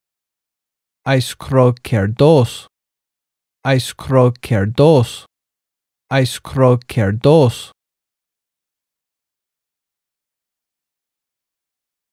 How to pronounce Aischrokerdōs in Biblical Greek - (αἰσχροκερδῶς / in fondness for dishonest gain)
αἰσχροκερδῶς (aischrokerdōs) means in fondness for dishonest gain in Biblical Greek.

This video shows how to pronounce αἰσχροκερδῶς in the Erasmian pronunciation system.

Example Bible Verses:

1 Peter 5:2
shepherd the flock of God among you, exercising oversight not by compulsion but willingly, in accordance with God, and not greedily but eagerly,